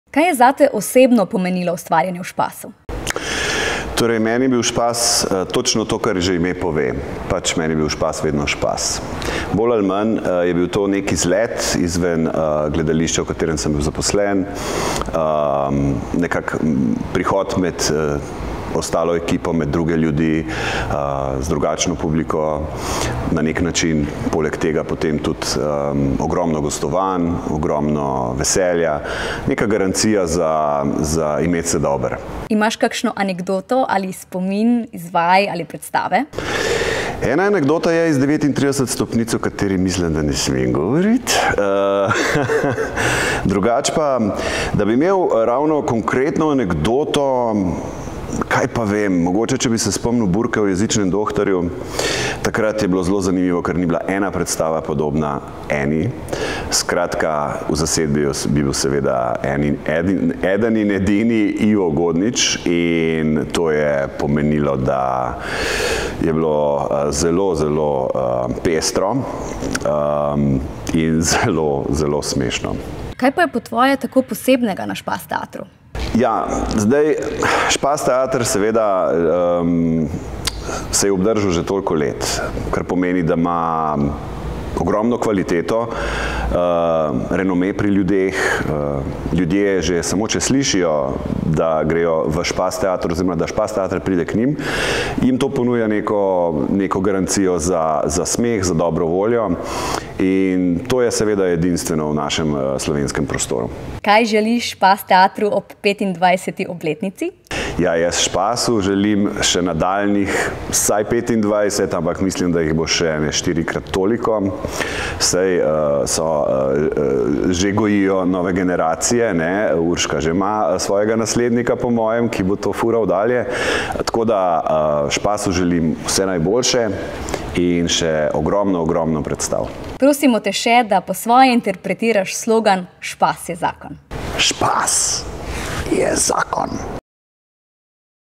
Uroš Smolej - intervju | ŠPAS TEATER praznuje 25 let
Uroš Smolej, igralec | 2022
V Špas teatru je igral v komedijah 39 stopnic, Do nazga!, Burka o jezičnem dohtarju.